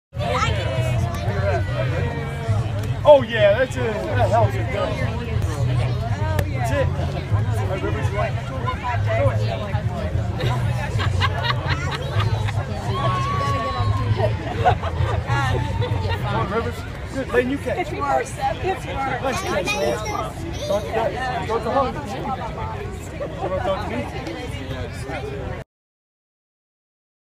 Dallas Cowboys TE Jason Witten Plays Catch With His Kids And Romo's Kids
Jason Witten reverses roles with Romo - not that Romo - as he plays catch with his kids and Tony Romo's kids.